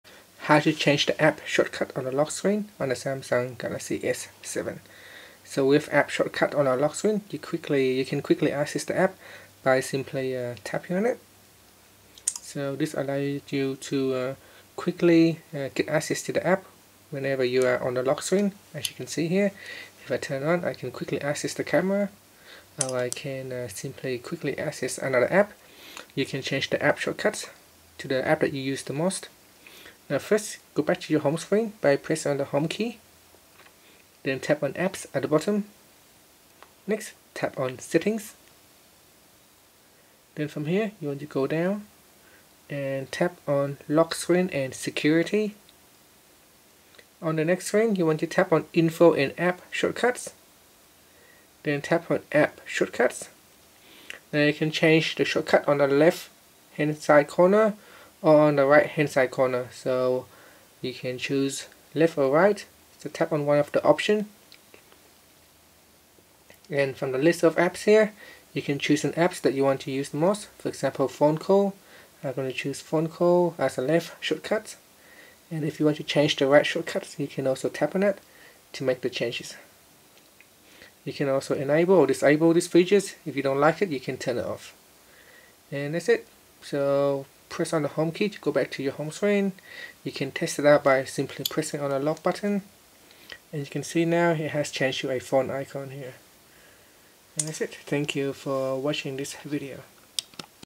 Samsung Galaxy S7: How to Change App Shortcuts on Lock Screen
Learn how you can change the app shortcuts on the lock screen on Samsung Galaxy S7.